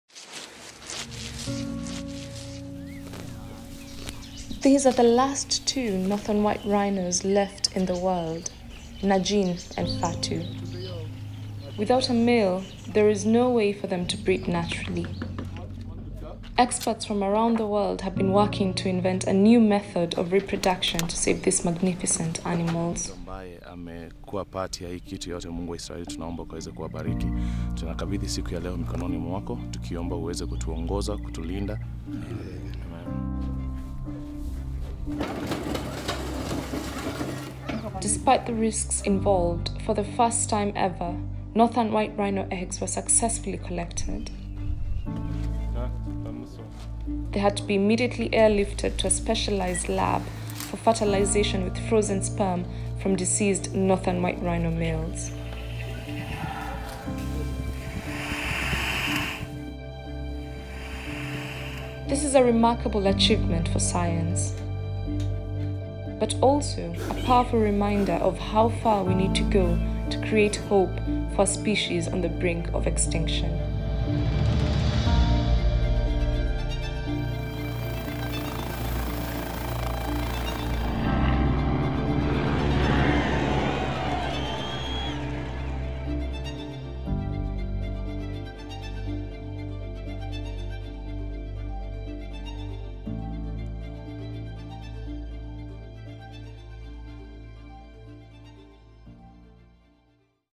Successful Egg Harvest Breaks New Ground in Saving the Northern White Rhino
There are only two northern white rhinos left worldwide, both of them female. Saving this representative of megafauna from extinction seems impossible under these circumstances, yet an international consortium of scientists and conservationists just completed a procedure that could enable assisted reproduction techniques to do just that. On August 22, 2019, a team of veterinarians successfully harvested eggs from the two females who live in Ol Pejeta Conservancy in Kenya — a procedure that has never been attempted in northern white rhinos before. The eggs will now be artificially inseminated with frozen sperm from a northern white rhino bull, and in the near future the embryo will be transferred to a southern white rhino surrogate mother. The successful procedure was a joint effort by the Leibniz Institute for Zoo and Wildlife Research (Leibniz-IZW) Berlin, Avantea, Dvůr Králové Zoo, Ol Pejeta Conservancy and the Kenya Wildlife Service (KWS).

Written by Ami Vitale, Sean Viljoen, Jan Stejskal, Steven Seet, Barbara de Mori and Jan Zwilling
Filmed by Sean Viljoen, Ami Vitale and Jan Zwilling
Edited by Sean Viljoen